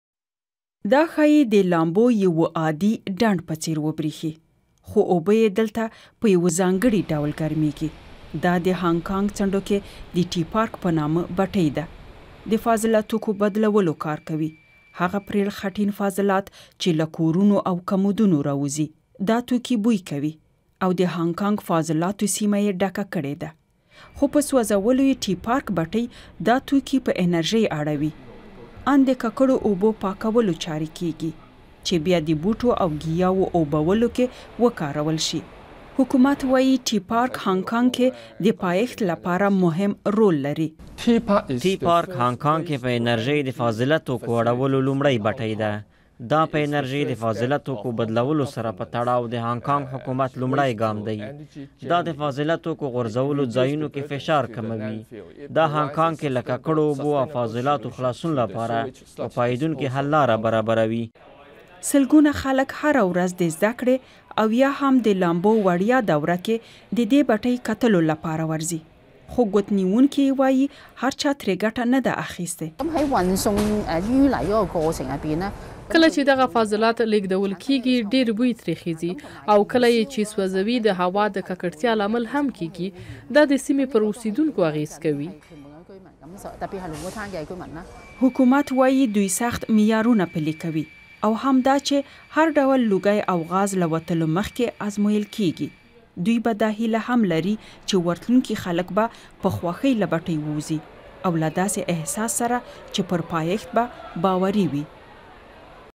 د انرژۍ د توليد نوې لار
په هانګ کانګ کې د خځلو په یوه بټۍ کې حمام پرانیستل شوی دی.
 حمام دلته د خلکو له فضله توکو برېښنا تولیدوي.
د دې ځای نوم ټي پارک دی دلته بې کاره توکي له منځي ځي او هم په کې خلک حمام کوي.
خو ځینې اندېښمن دي چې دا ځای به پر چاپیریال څه اغېز وکړي.